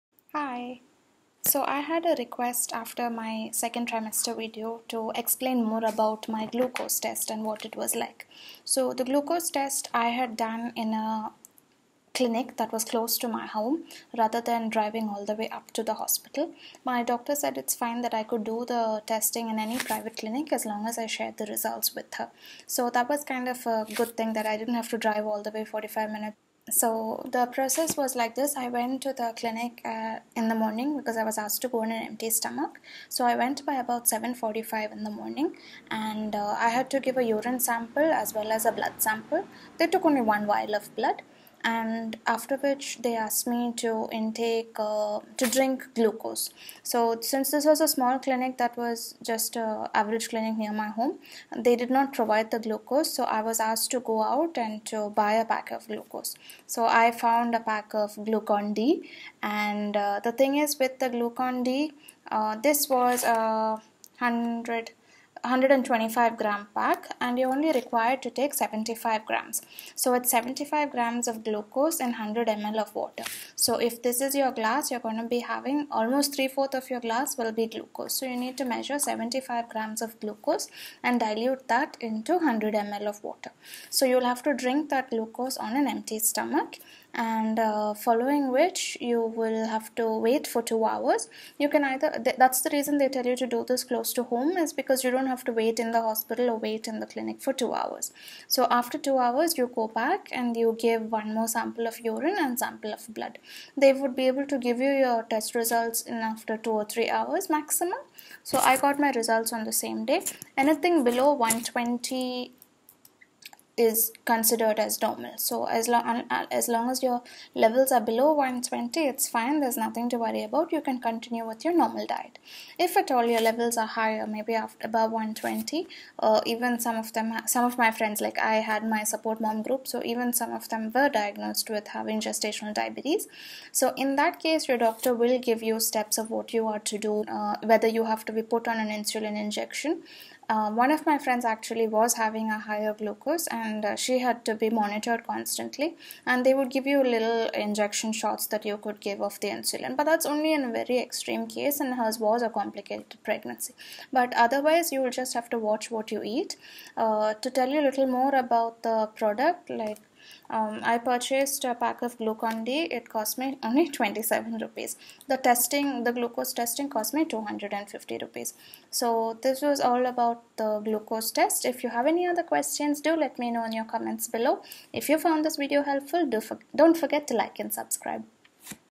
Glucose Test during Pregnancy
Glucose Test how its done in India . What to expect? What the normal level ? What happens if its high? How to do prepare your glucose test at home